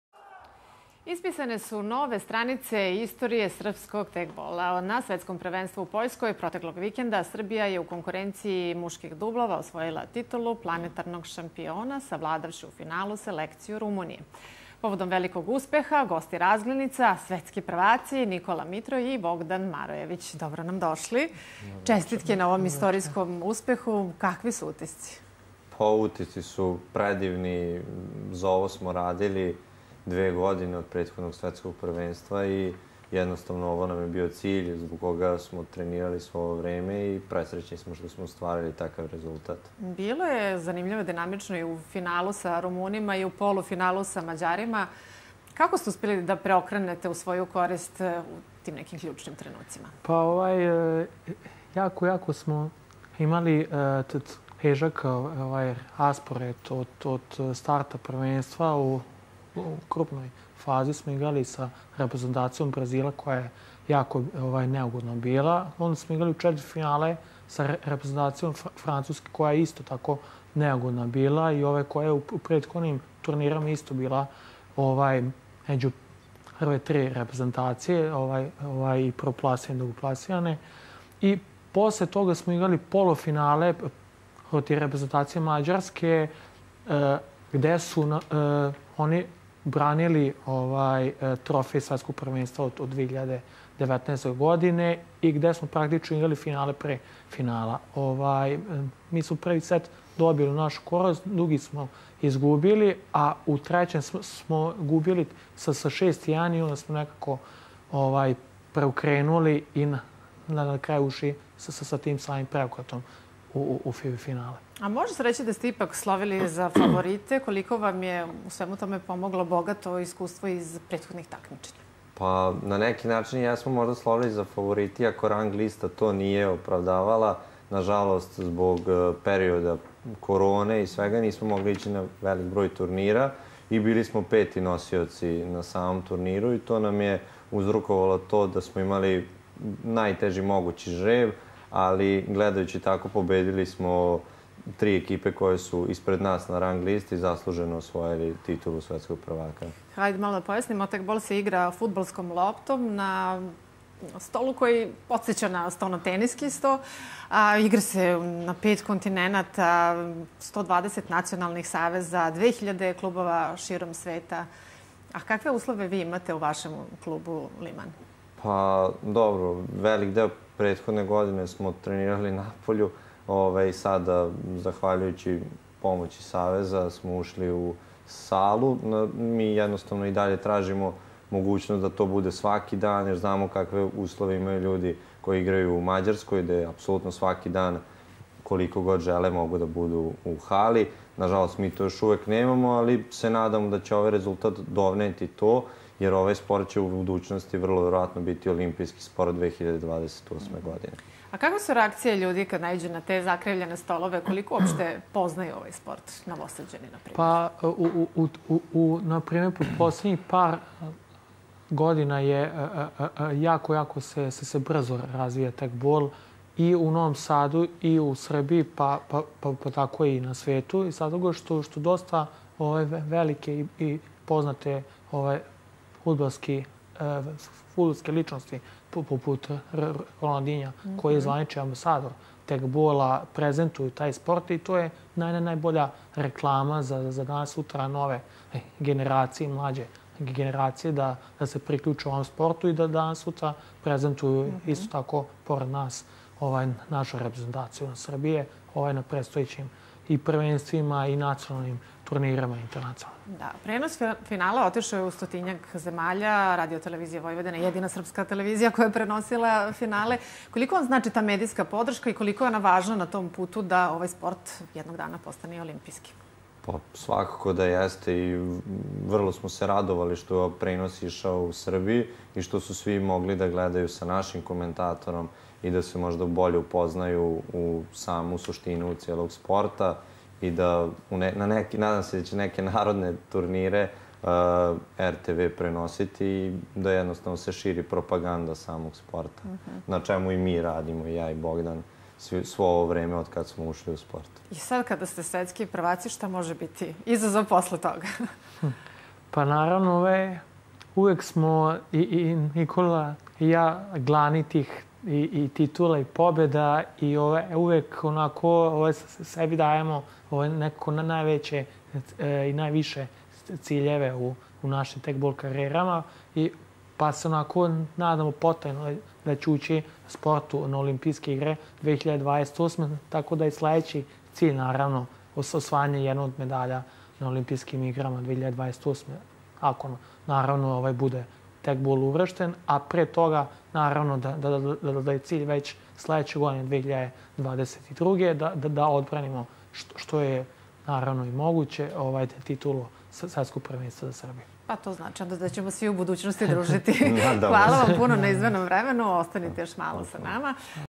Ispisane nove stranice istorije srpskog tekbola
Na Svetskom prvenstvu u Poljskoj, proteklog vikenda, Srbija je u konkurenciji muških dublova osvojila titulu planetarnog šampiona, savladavši u finalu selekciju Rumunije. Povodom velikog uspeha, gosti Razglednica svetski prvaci, Nikola Mitro i Bogdan Marojević.